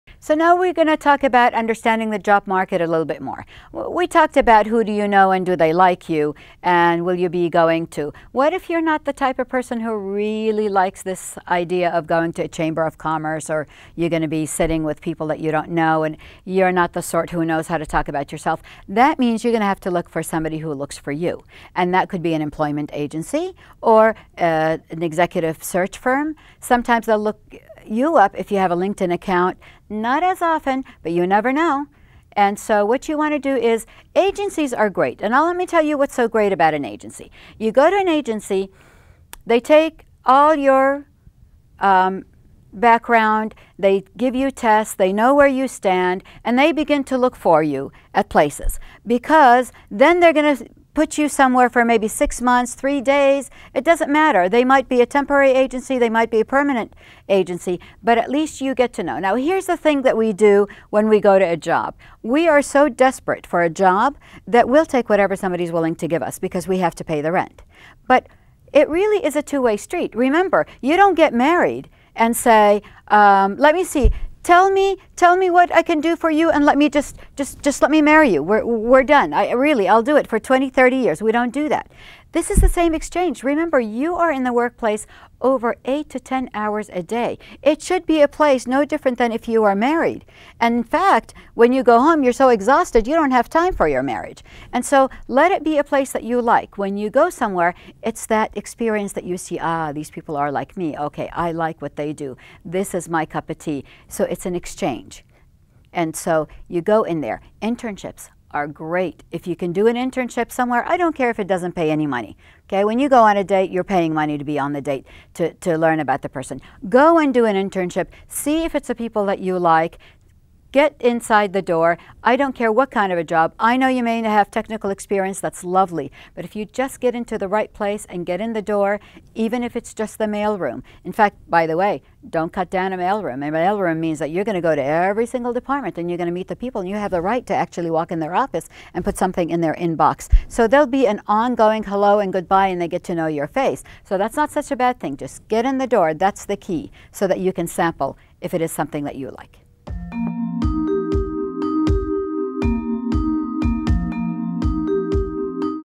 #2 Understanding the Job Market
How to Get a Job Video Series
Hosted by Eva Bitar, Adjunct Faculty at West Los Angeles College

Go West. Go Far.